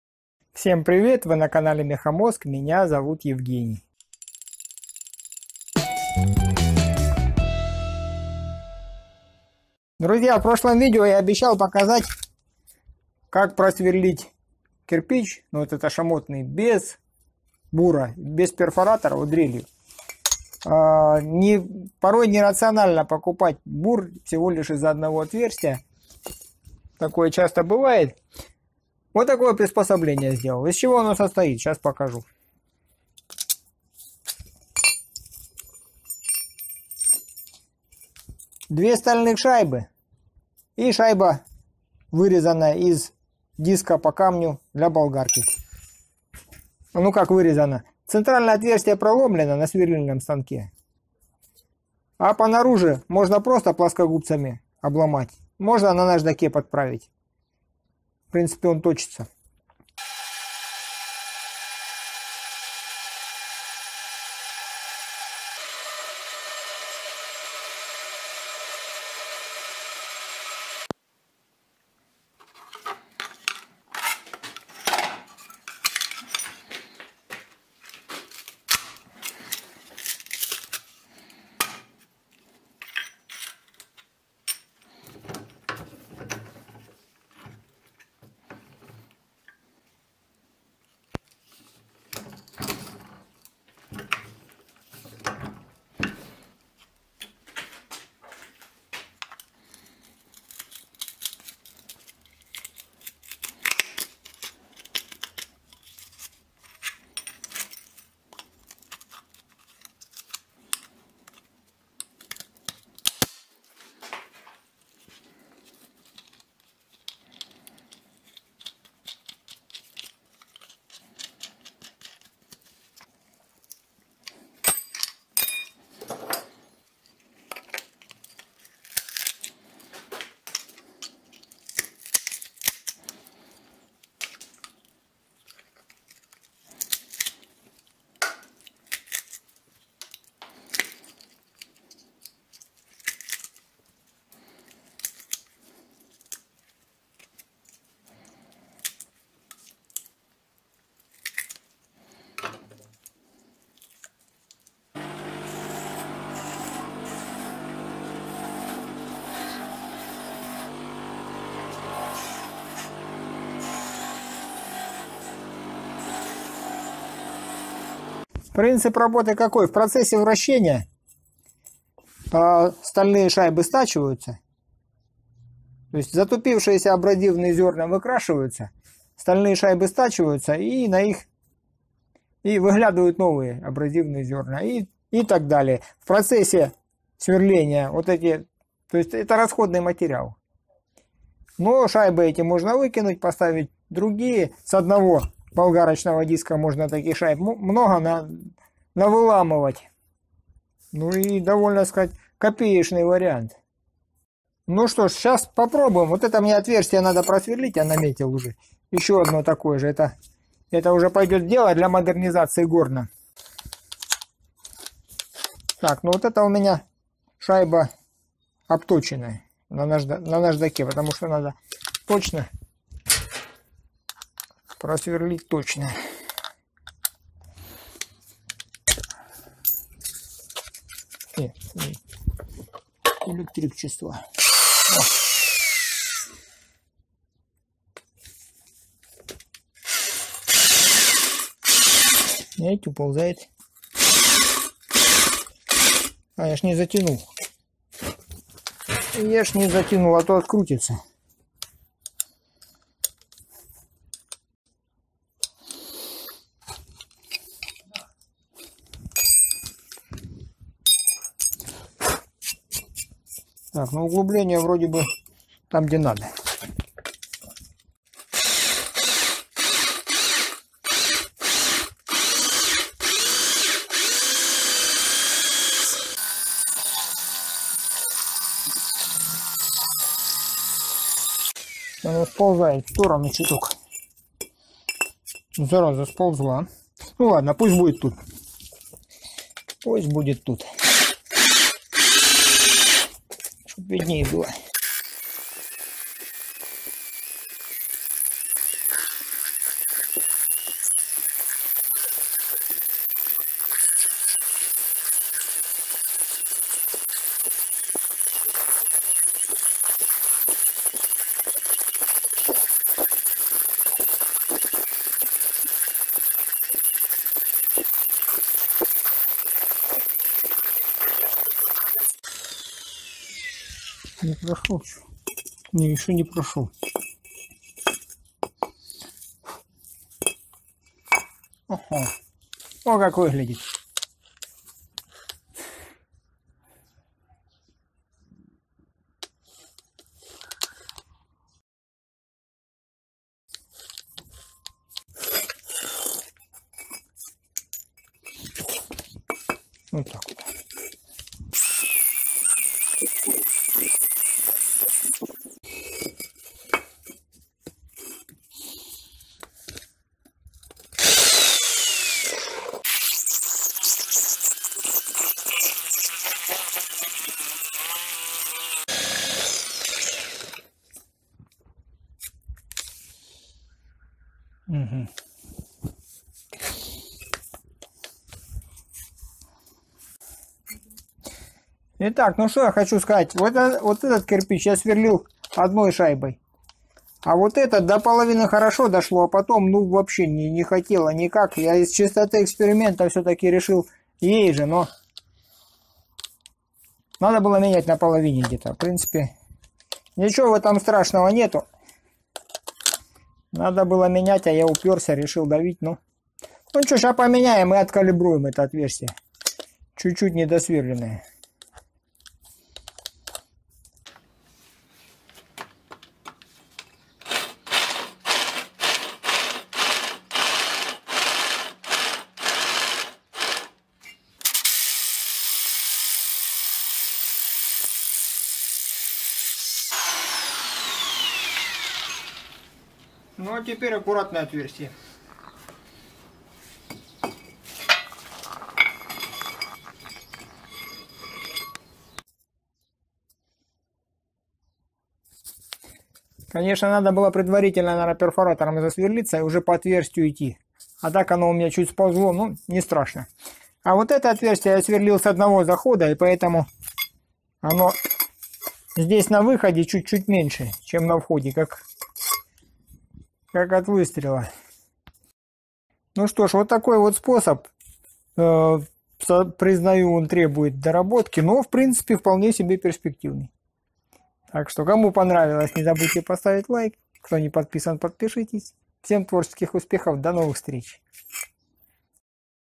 Бур по бетону своими руками
В этом видео я показываю способ, как без бура просверлить кирпич.

For channel development:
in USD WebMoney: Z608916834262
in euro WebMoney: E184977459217